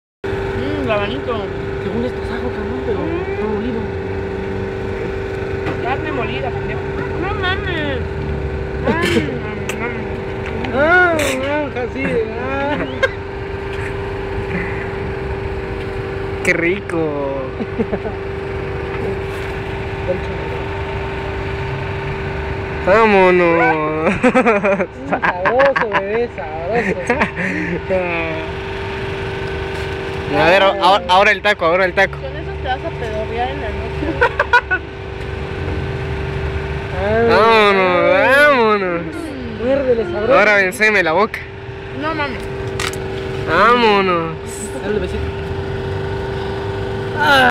cenando tacos afuera de fabricas
aki encontramos a hassive y a dafne saboreando sus ricos tacos jajaja